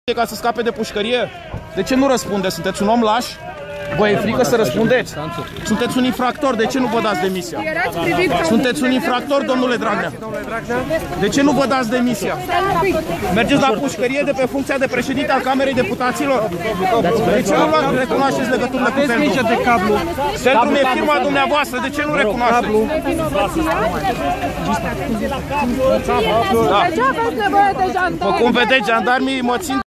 Cat de laș e Liviu Dragnea. Nu are curaj să zică nimic lângă mine
Liviu Dragnea a venit la termenul de la Înalta Curte de Casație și Justiție unde este judecat pentru că a furat din banii orfanilor din Teleorman.  L-am prins la întrare,  dar a refuzat iar sa vorbească.